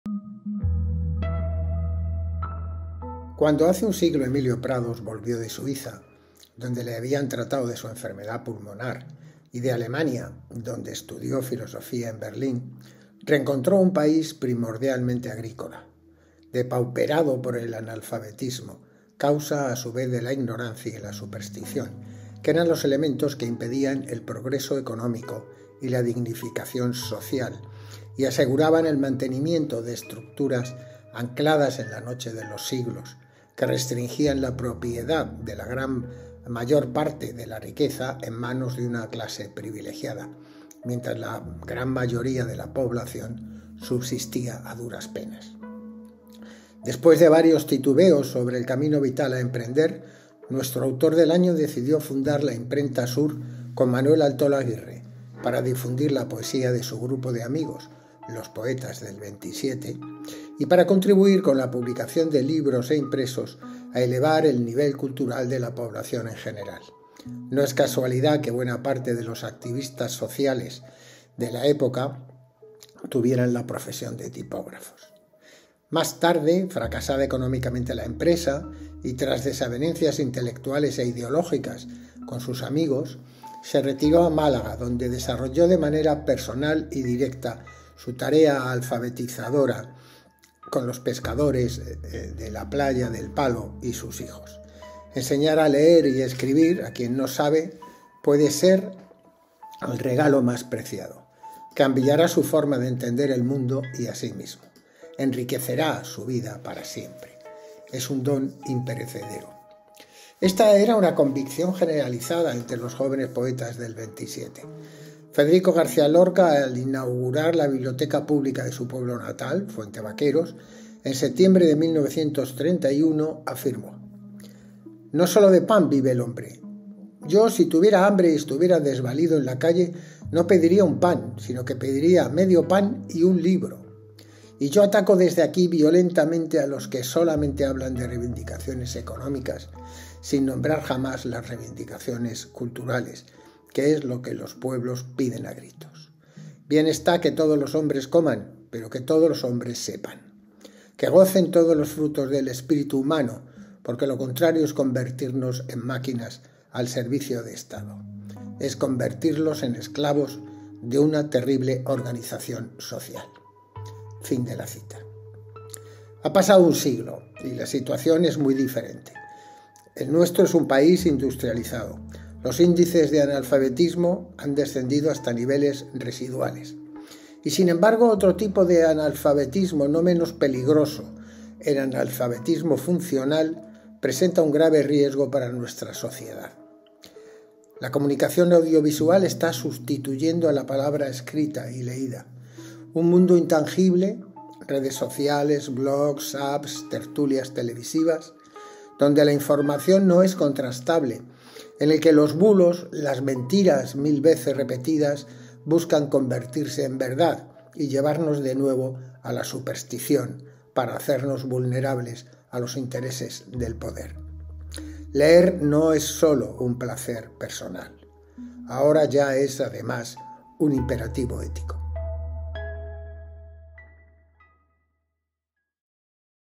Día de la Lectura 2021 - Alocución Ciudadana
Desde el Centro Andaluz de las Letras, se invita a la lectura, en los centros educativos, bibliotecas públicas, clubes de lectura e instituciones culturales y literarias, de la alocución ciudadana elaborada por el catedrático de Literatura Española Julio Neira, titulada ‘Manifiesto de la Lectura en la sociedad audiovisual’. Sus emotivas palabras retratan la vida y obra de Emilio Prados y su relación con los poetas de la Generación del 27: “Enseñar a leer y escribir a quien no sabe puede ser el regalo más preciado. Cambiará su forma de entender el mundo y a sí mismo. Enriquecerá su vida para siempre. Es un don imperecedero. Esta era una convicción generalizada entre los jóvenes poetas del 27”, escribe Julio Neira en la alocución ciudadana. El texto completo puede descargarse gratuitamente desde la web del Centro Andaluz de las Letras.